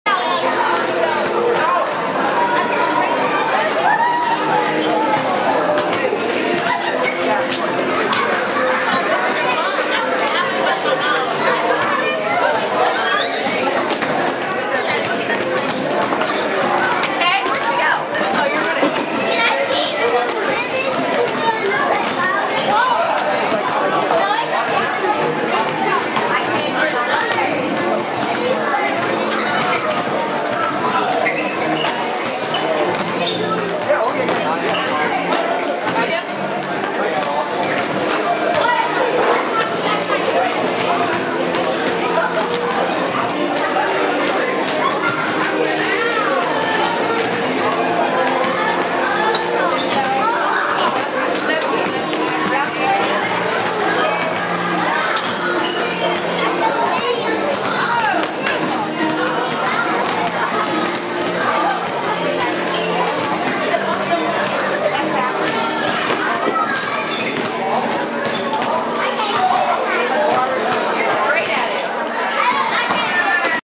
Go NOLA '10 FABC, Crossroads Cincinnati Girls playing in Gym
Some girls with some sort of jumping twirling thing.
VBS = Vacation Bible School, FABC = Franklin Avenue Baptist Church
A little recording in the Gym, there's a walking track above on the perimeter where I took more video.